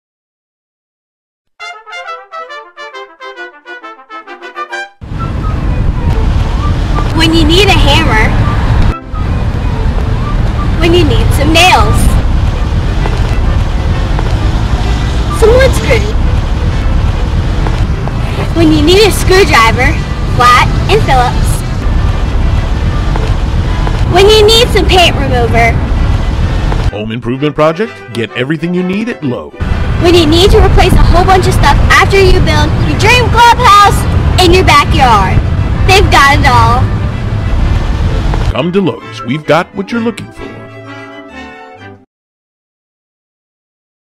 Creative Kids: Dream Clubhouse
Mock TV commercial for hardware store. Kiley should not have used (lost) Dad's tools. Satire. Spoof. TV commercial satire.